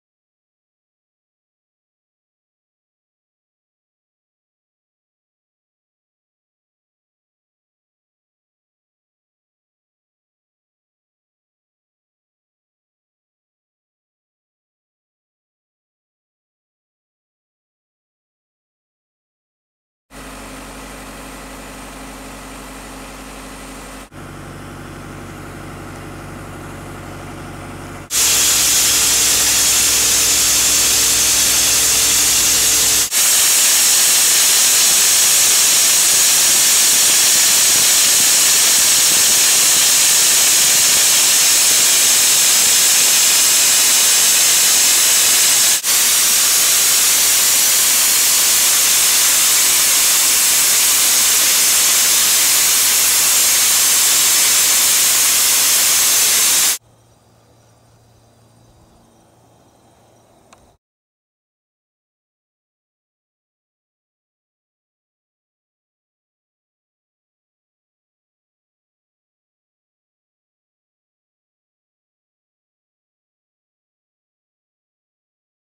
Sodablasting yacht Antifoul removal.
Soda Blasting services ltd Removing anti foul from yacht
BLASTIT.co.nz